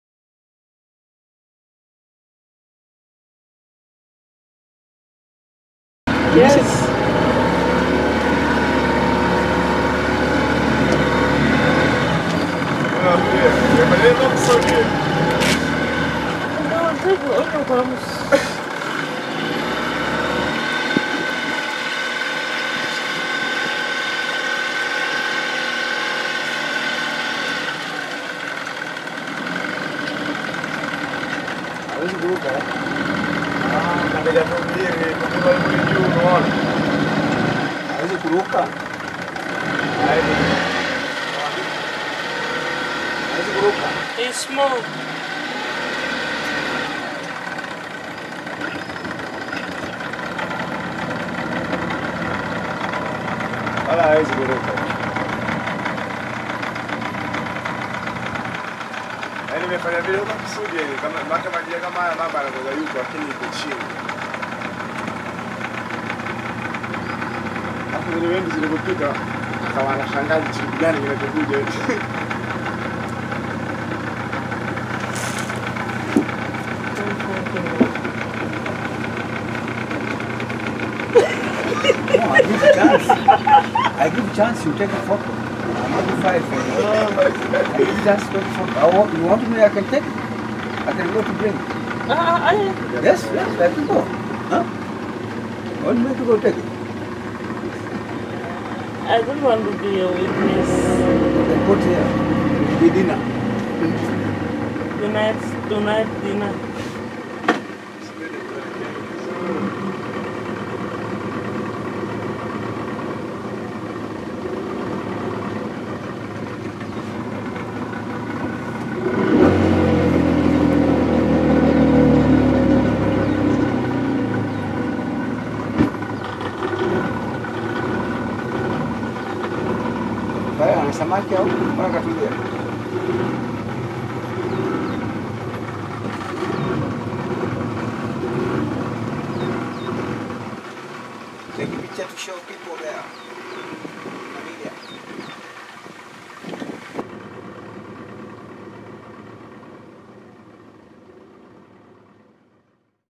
Pangani River cruise - crocodile
A crocodile seen on our boat cruise on the Pangani River, in Tanzania.